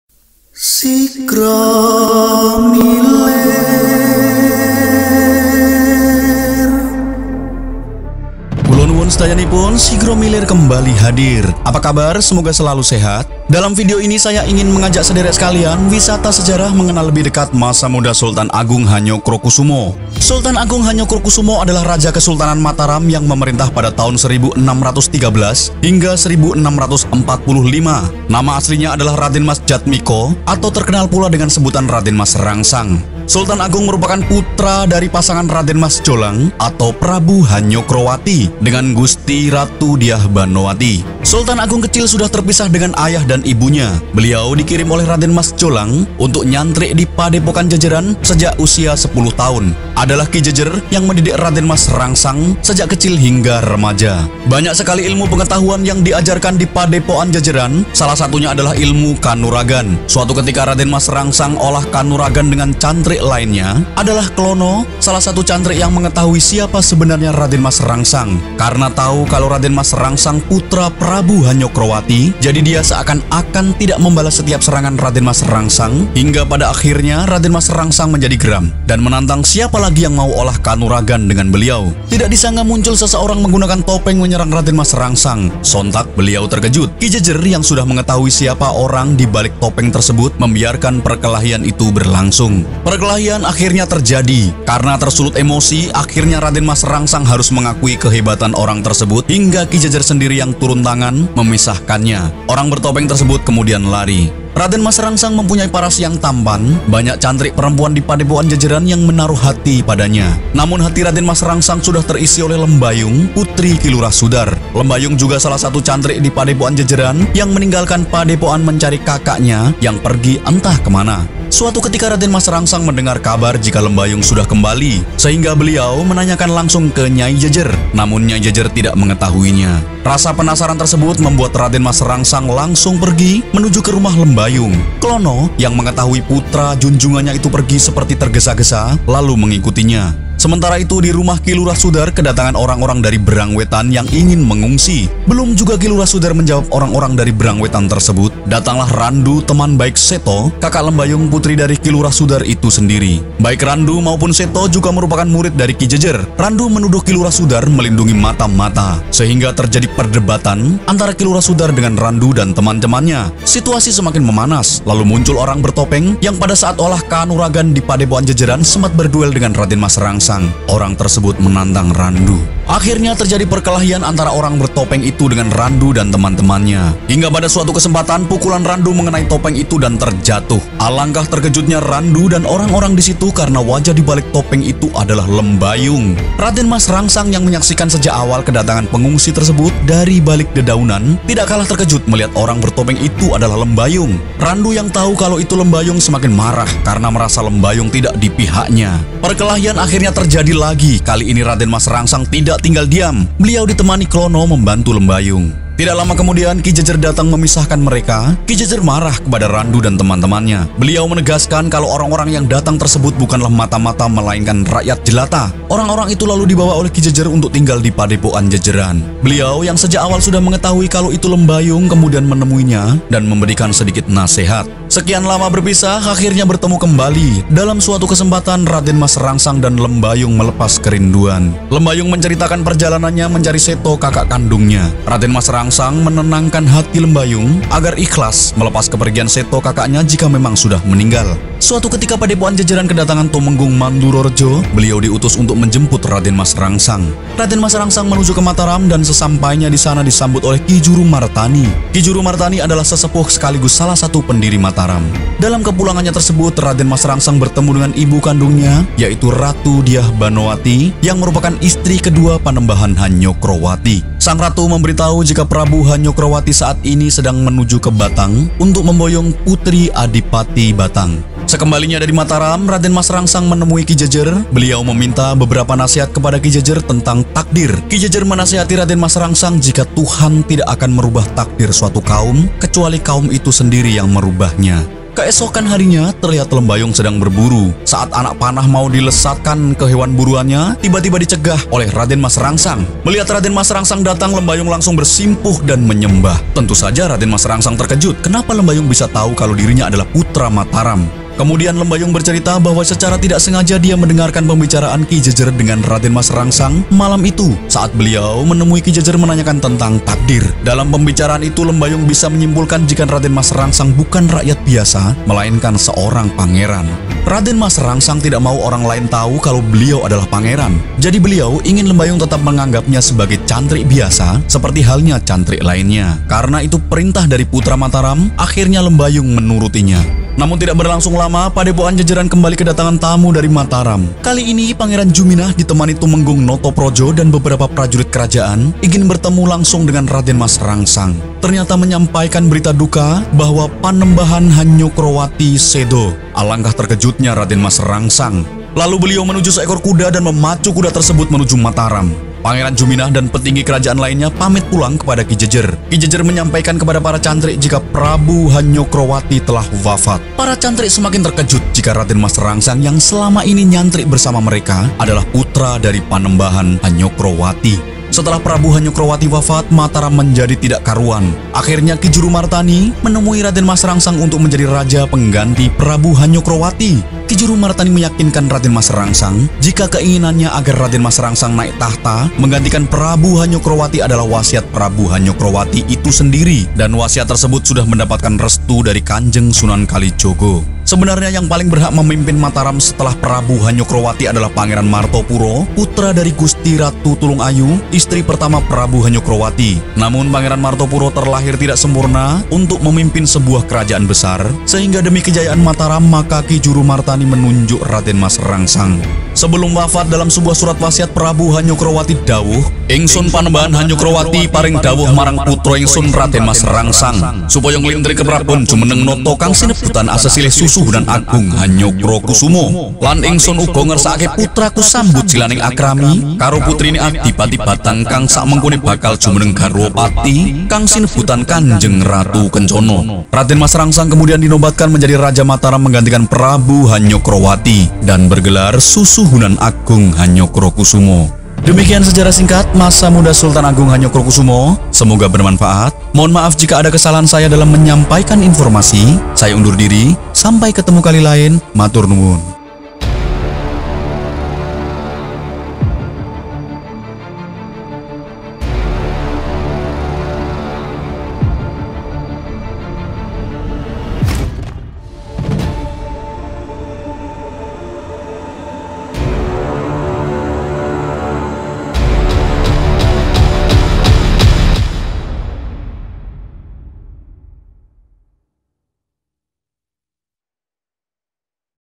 Cerita Bergambar Kisah Sultan Agung Hanyokrokusumo muda hingga dinobatkan menjadi Raja Mataram
Sultan Agung Hanyokrokusumo adalah raja Kesultanan Mataram yang memerintah pada tahun 1613 hingga 1645. Nama aslinya adalah Raden Mas Jatmika, atau terkenal pula dengan sebutan Raden Mas Rangsang. Sultan Agung merupakan putra dari pasangan Raden Mas Jolang atau Prabu Hanyokrowati dengan Gusti Ratu Dyah Banowati.
Sultan Agung kecil sudah terpisah dengan ayah dan ibunya. Beliau dikirim oleh Raden Mas Jolang untuk nyantrik di Padepokan Jejeran sejak usia 10 tahun, adalah Ki Jejer yang mendidik Raden Mas Rangsang sejak kecil hingga remaja.

Bagaimana kisahnya, yuk simak videonya.

Suatu ketika Padepokan Jejeran kedatangan Tumenggung Mandurorejo, beliau diutus untuk menjemput Raden Mas Rangsang.
Raden Mas Rangsang menuju ke Mataram dan sesampainya di sana disambut oleh Ki Juru Martani. Ki Juru Martani adalah sesepuh sekaligus salah satu pendiri Mataram.
Dalam kepulangannya tersebut Raden Mas Rangsang bertemu dengan ibu kandungnya yaitu Ratu Dyah Banowati yang merupakan istri kedua Panembahan Hanyokrowati. Sang Ratu memberitahu jika Prabu Hanyokrowati saat ini sedang menuju ke Batang untuk memboyong putri Adipati Batang.

Raden Mas Rangsang kembali ke Jejeran, Namun tidak berlangsung lama, Padepokan Jejeran kembali kedatangan tamu dari Mataram. Kali ini Pangeran Juminah ditemani Tumenggung Notoprojo dan beberapa prajurit kerajaan ingin bertemu langsung dengan Raden Mas Rangsang. Ternyata menyampaikan berita duka bahwa Panembahan Hanyokrowati sedo. Alangkah terkejutnya Raden Mas Rangsang.
Lalu beliau menuju seekor Kuda dan memacu Kuda tersebut menuju Mataram. Pangeran Juminah dan petinggi kerajaan lainnya pamit pulang kepada Ki Jejer. Ki Jejer menyampaikan kepada para cantrik jika Prabu Hanyokrowati telah wafat. Para cantrik semakin terkejut jika Raden Mas Rangsang yang selama ini nyantrik bersama mereka adalah putra dari Panembahan Hanyokrowati.
Setelah Prabu Hanyokrowati wafat, Mataram menjadi semakin tidak karuan. Akhirnya Ki Juru Martani menemui Raden Mas Rangsang untuk menjadi Raja menggantikan Prabu Hanyokrowati. Ki Juru Martani meyakinkan Raden Mas Rangsang, jika keinginannya agar raden Mas Rangsang naik tahta menggantikan Prabu Hanyokrowati adalah wasiat Prabu Hanyokrowati itu sendiri, dan wasiat tersebut sudah mendapatkan restu dari Kanjeng Sunan Kalijaga.
Sebenarnya yang paling berhak memimpin Mataram setelah Prabu Hanyokrowati adalah Pangeran Martopuro putra dari Gusti Ratu Tulung Ayu, istri pertama Prabu Hanyokrowati. Namun Pangeran Martopuro terlahir tidak sempurna untuk memimpin sebuah kerajaan besar, sehingga demi kejayaan Mataram maka Ki Juru Martani menunjuk Raden Mas Rangsang.
Sebelum wafat, dalam sebuah surat wasiat Prabu Hanyokrowati dhawuh “Ingsun Panembahan hanyokrowati paring dhawuh marang putro ingsun Raden Mas Rangsang, supoyo nglintir keprabon jumeneng noto kang sinebutan asesilih Susuhunan Agung Hanyokrokusumo, lan ingsun ugo ngersakake putraku sambut silaning akrami karo putrine adipati Batang kang sak mengkone bakal jumeneng garwo pati kang sinebutan kanjeng Ratu Kencono”
Raden Mas Rangsang kemudian dinobatkan menjadi Raja Mataram menggantikan Prabu Hanyokrowati, dan bergelar Susuhunan Agung Hanyokrokusumo.